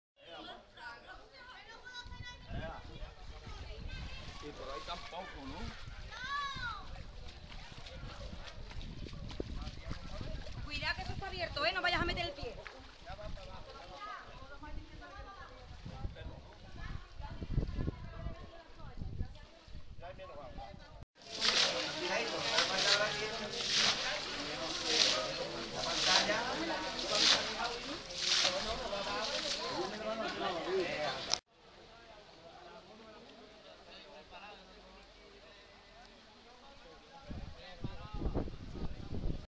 Anegaciones en Utrera por una espectacular tormenta con lluvia, granizo, viento y aparato eléctrico
La noche del 25 de abril ha dejado en Utrera una espectacular tormenta con lluvia, granizo, viento y aparato eléctrico, dejando algunas anegaciones.